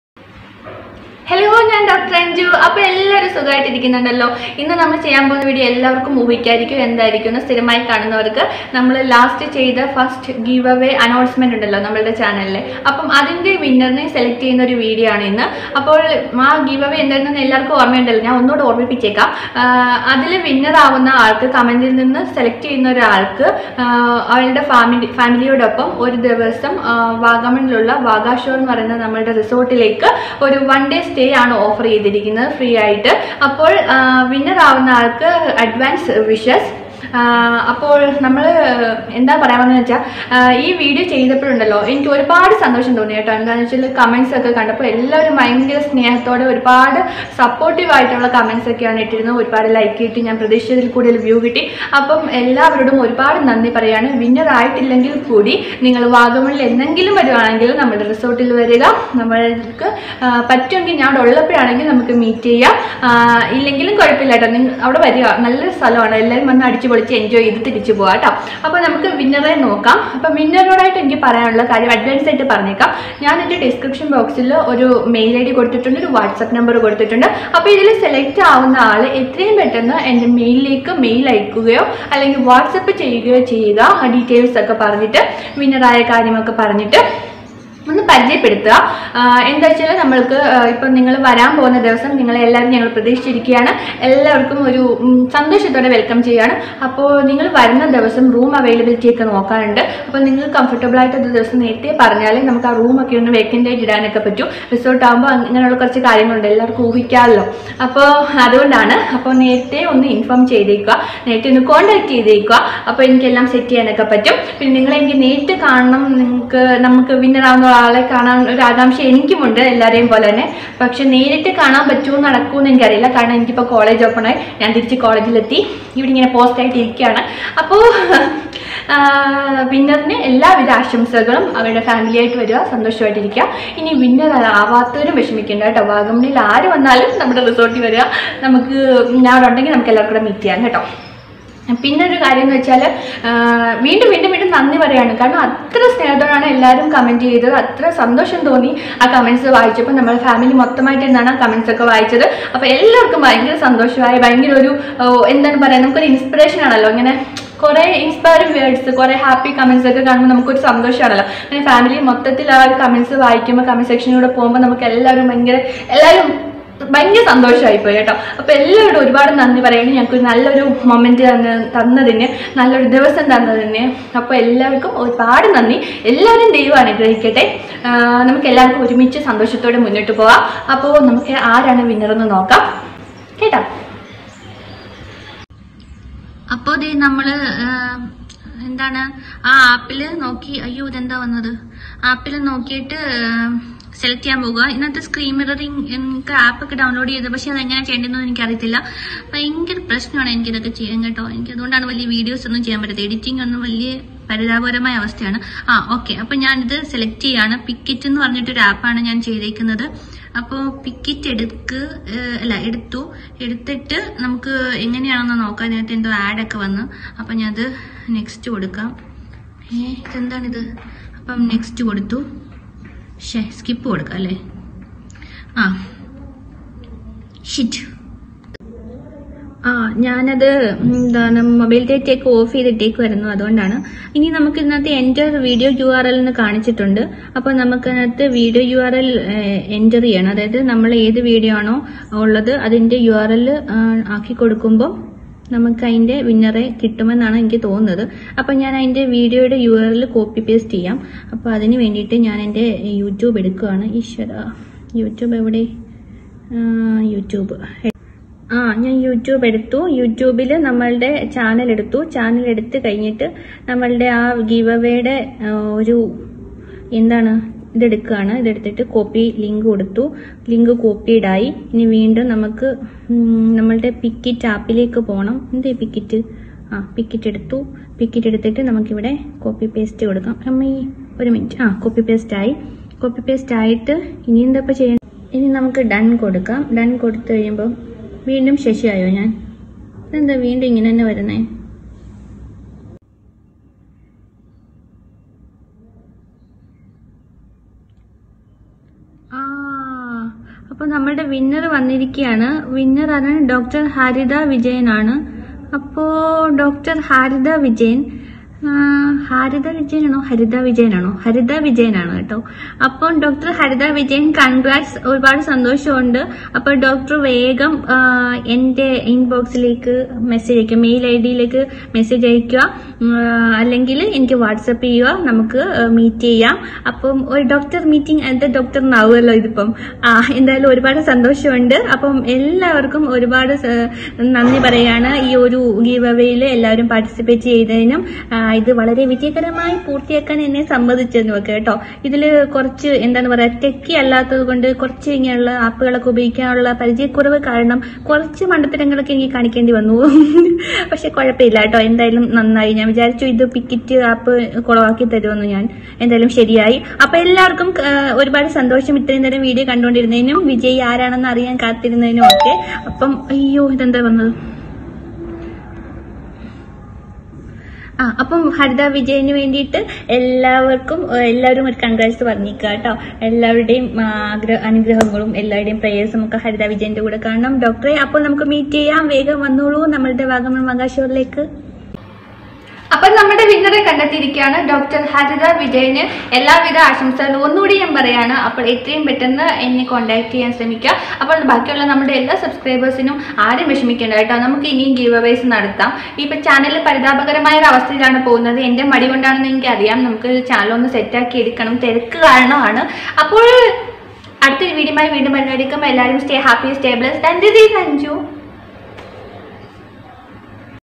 Giveaway Winner 🏆 Let's Celebrate ❤️🥰
Hey❤️❤️❤️

Hi My Dearest Winner 🤝
Congratulations!!
we would like to offer you a one day free stay in our own Resort-"Vagamon Vagashore"
Welcoming you and your family 👍🙏